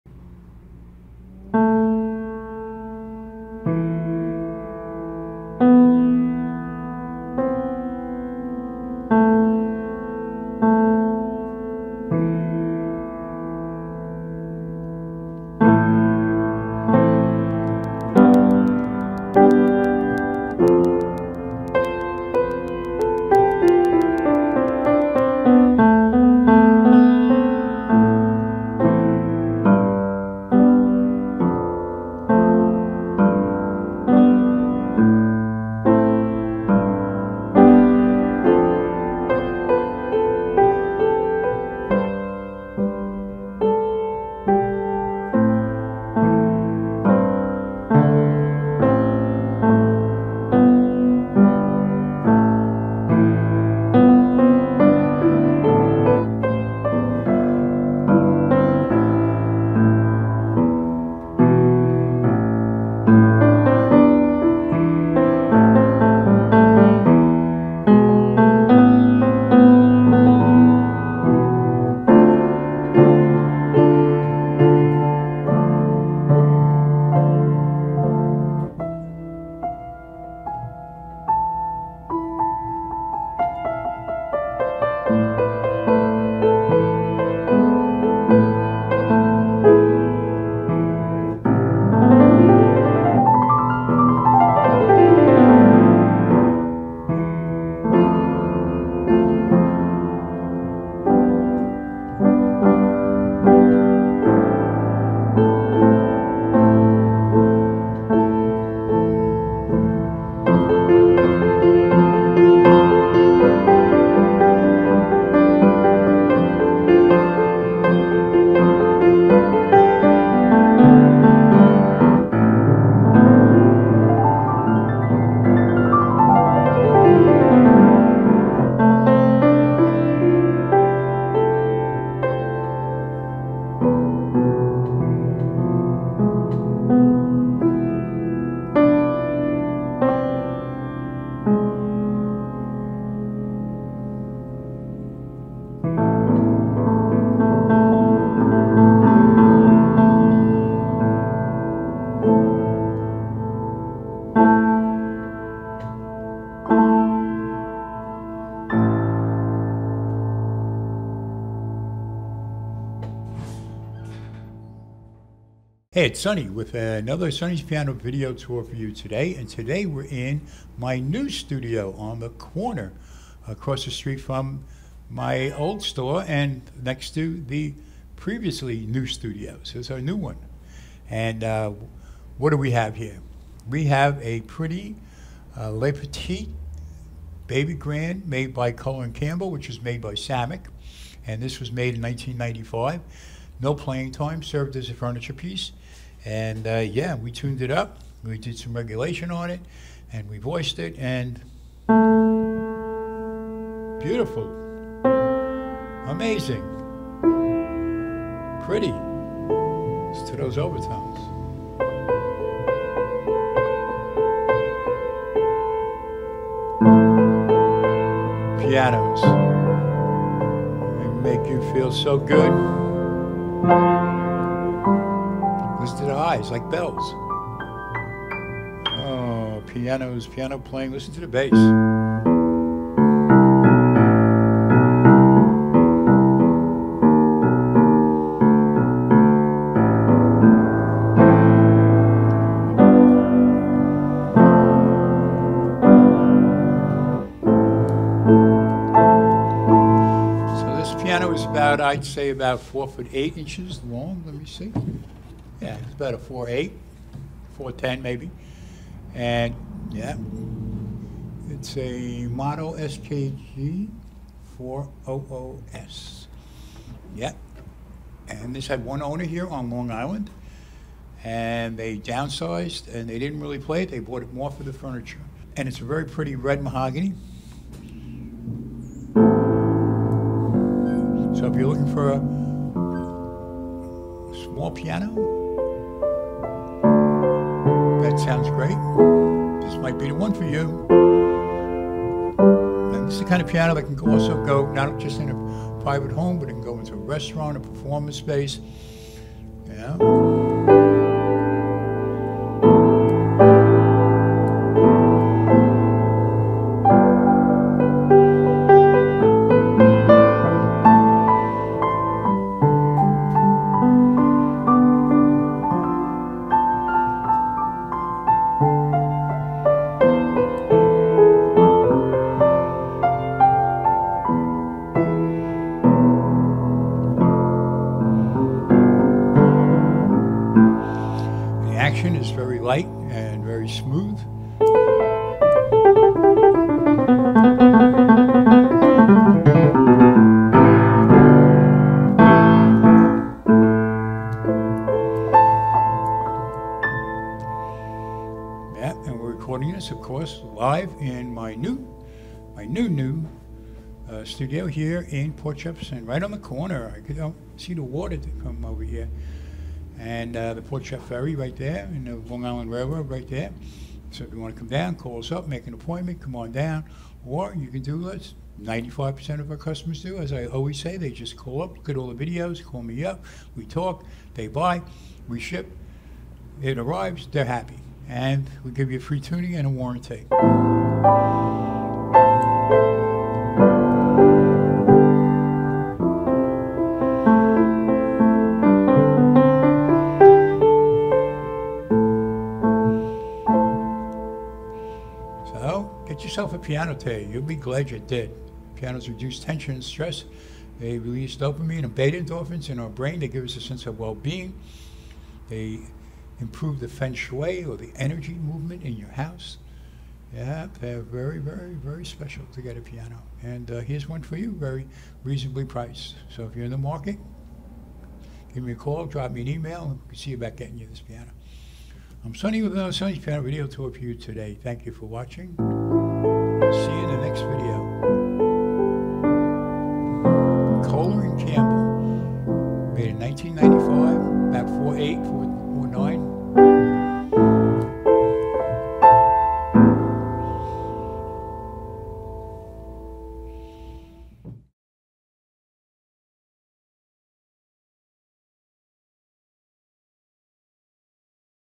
Kohler & Campbell Le Petit Baby Grand, FOR SALE! 4'8" 1995, Hi-Gloss Red Mahogany Excellent $3950
Kohler & Campbell, le petit baby grand, red mahogany Hi-gloss, 4'8". One owner, very little playing time. Crisp, clear, rich tone. If you want a baby grand but don't have a lot of room this is the piano for you. Kohler & Campbell is made by Samick Piano Company one of the largest piano manufacturers in the world. Nationwide delivery, free after delivery tuning. Questions?